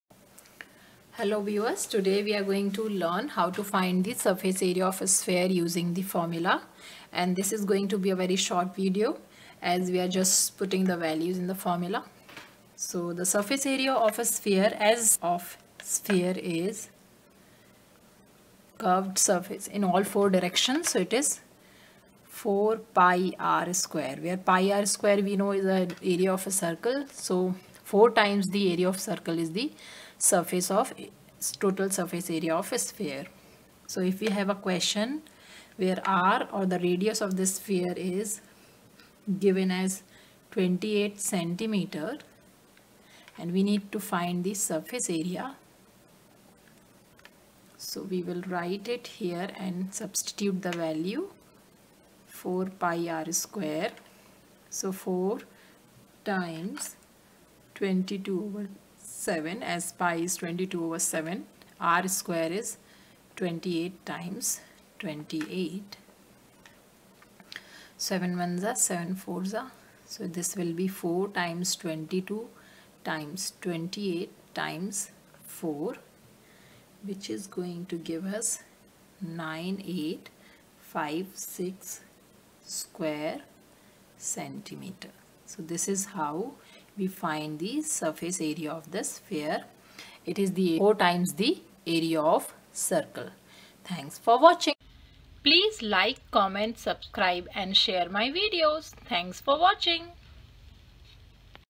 HOW TO FIND THE SURFACE AREA OF A SPHERE
This is a video showing  how to find the surface area of a sphere using formula.